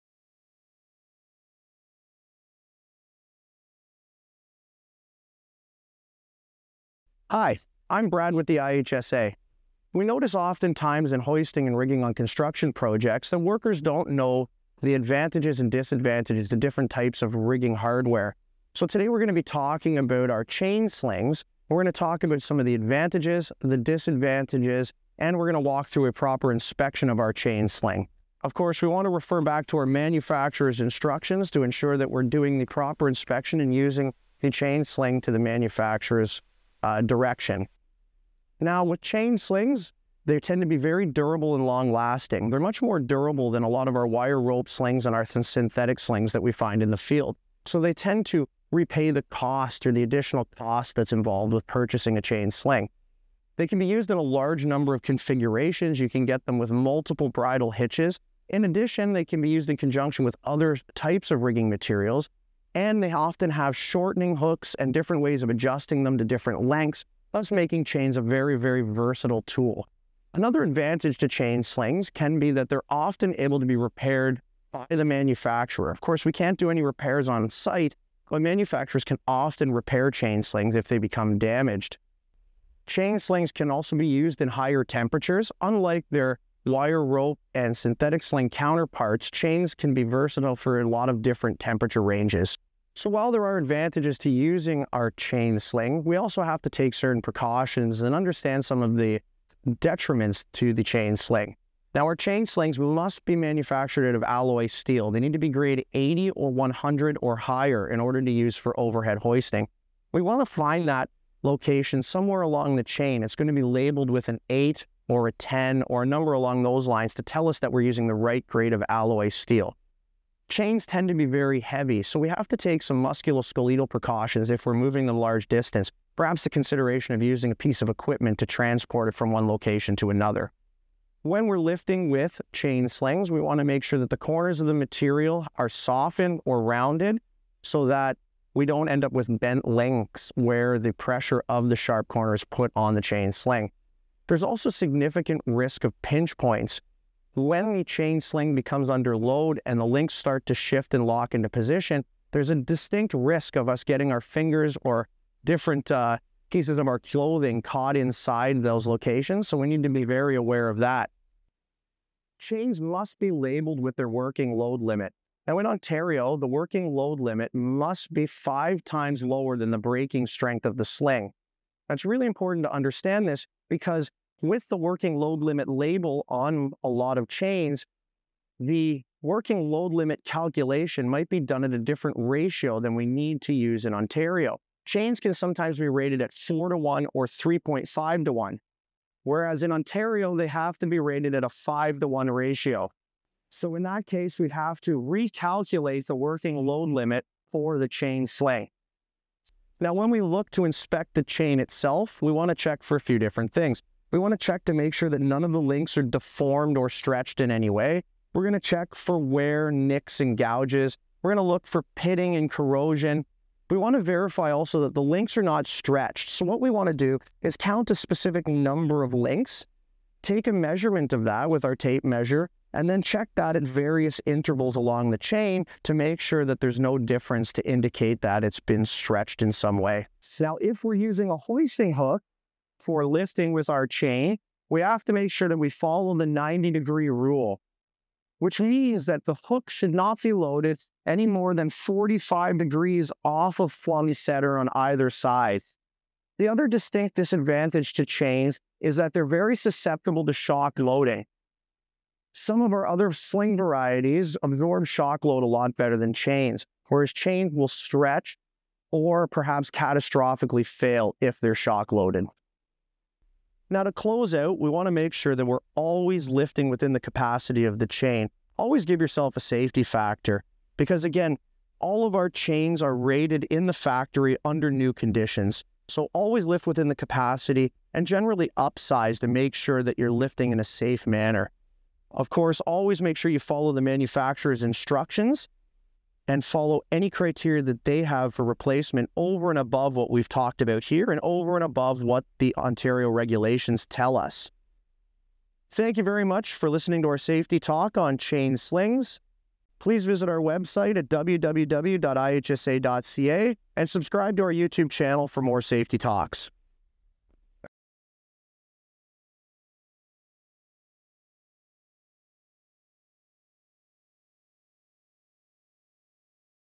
Chain slings: A general overview
When it comes to hoisting and rigging on construction projects, it’s important to understand the pros and cons of the different rigging hardware options and the proper way to inspect them. This Safety Talk video discusses both the benefits and drawbacks of using a chain sling, and provides guidance on the step-by-step inspection process for ensuring the safety and functionality of a chain sling.

Watch this video and learn more.

0:00 - Introduction
0:38 - Advantages of working with chain slings
1:33 - Precautions for working with chain slings
3:20 - Inspecting the chain sling
3:50 - Using a hoisting hook
4:06 - Danger of shock loading
4:24 - Lifting within the capacity of the chain sling
4:55 - Accessing additional resources